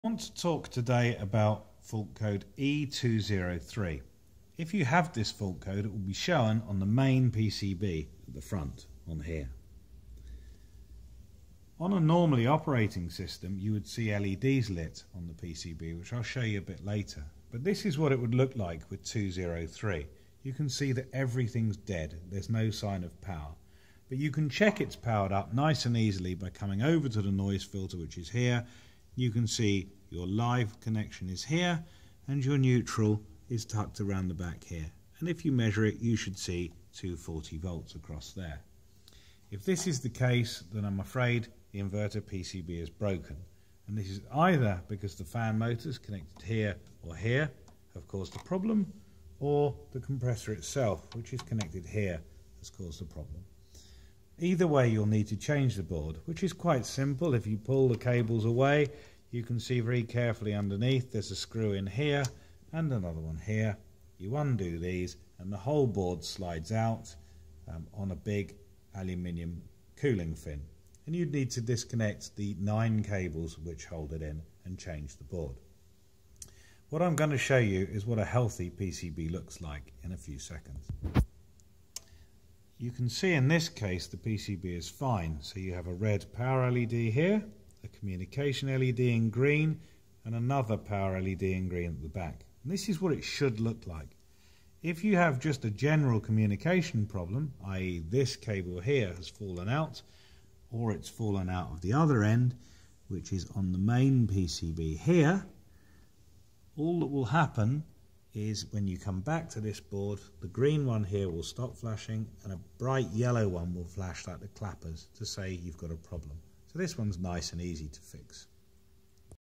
E203 error Samsung air source heat pumps AE090JXYDEH AE160JXYDEH
How to fix E203 fault on Samsung heat pumps and aircon unit with the controller shown Gen 5